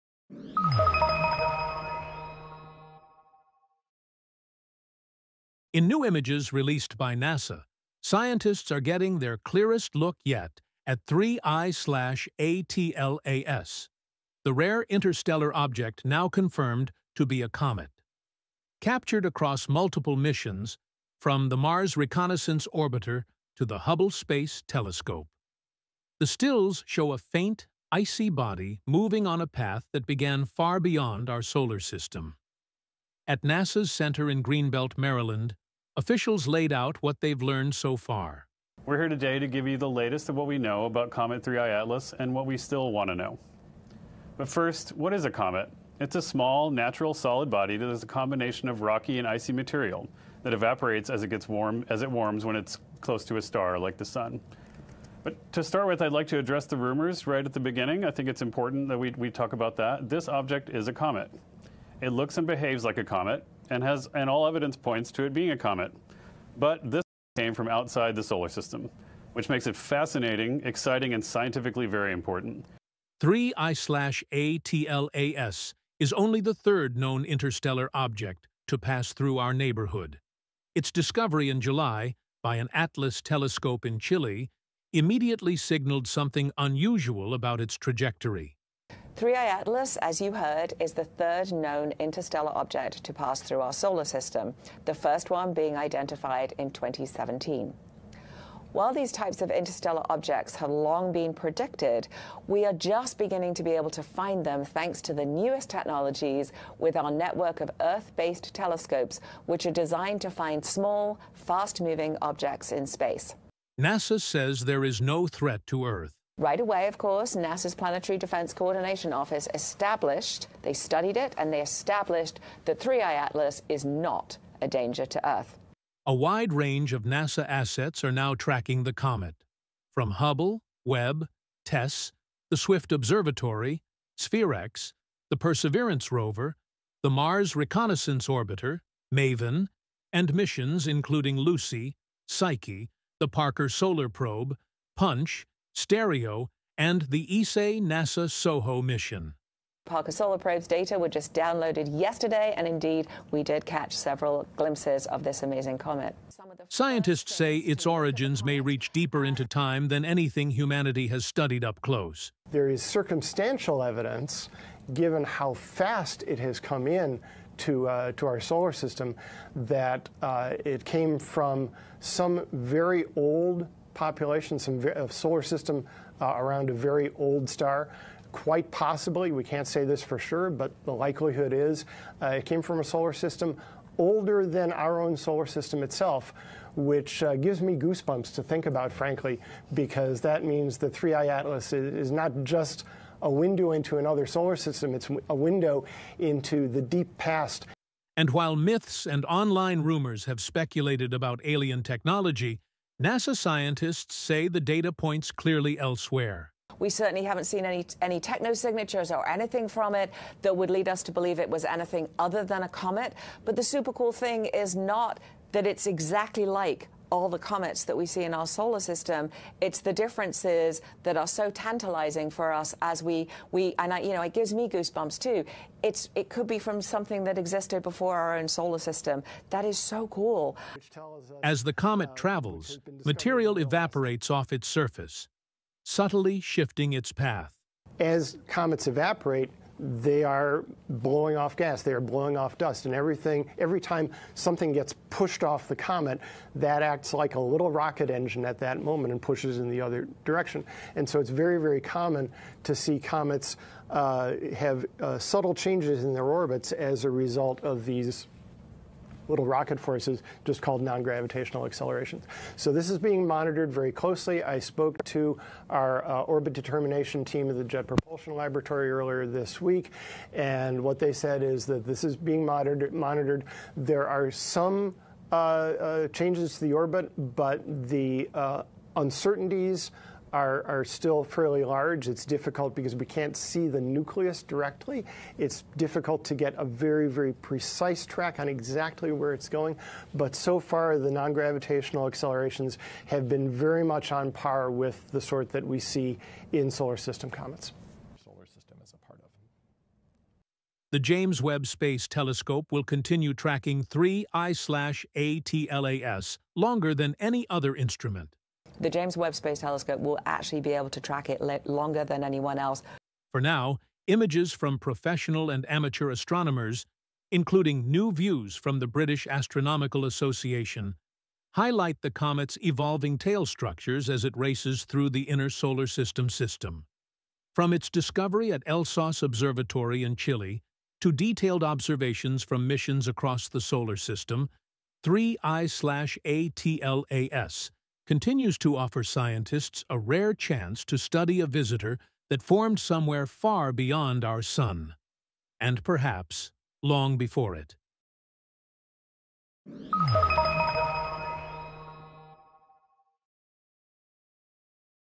NASA Unveils New Images of Interstellar Comet 3I/ATLAS
In a major breakthrough, NASA has released the clearest images yet of 3I/ATLAS — an interstellar comet that originated outside our solar system. Captured by a fleet of missions including the Hubble Space Telescope, Mars Reconnaissance Orbiter, and even the James Webb Space Telescope, this icy traveller is only the third known object from beyond our solar system to pass through it.

Scientists believe 3I/ATLAS could date back to a solar system older than our own, offering unprecedented insights into the building blocks of the cosmos. Join us as we explore what NASA has learned so far, what mysteries remain, and why this rare visitor is thrilling astronomers around the world.